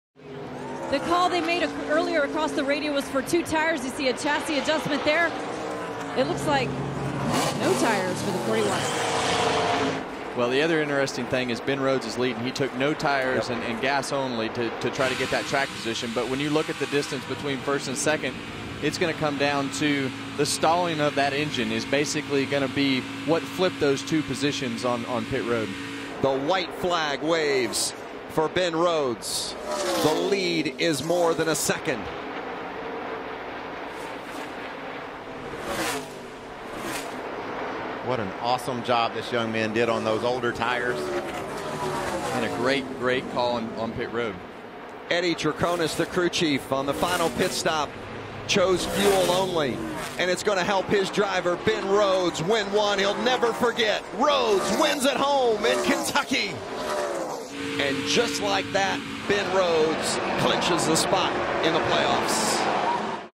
Final Laps: Bold pit-road call sends Kentucky native Rhodes to victory
Ben Rhodes' crew chief Eddie Troconis made the bold call to take no new tires on the final pit stop, and it propelled Rhodes to victory in Kentucky.